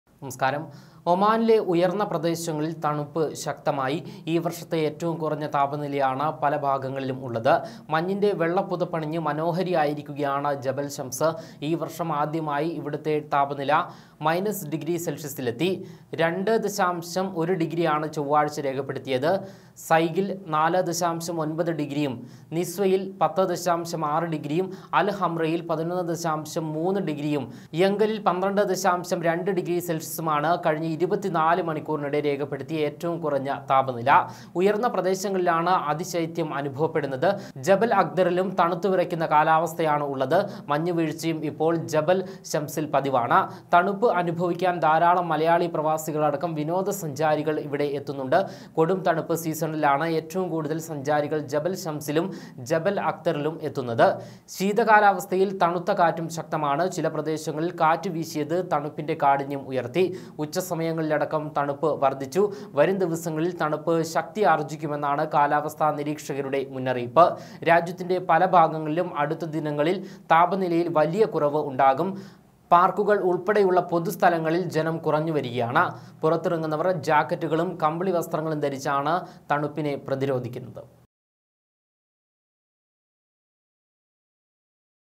ഒമാൻ തണുത്തു വിറക്കുന്നു... താപനില മൈനസിൽ | oman snowfall
തണുത്തു വിറച്ച് ഒമാൻ...
താപനില മൈനസ് ഡിഗ്രി
സെല്‍ഷ്യസിലെത്തി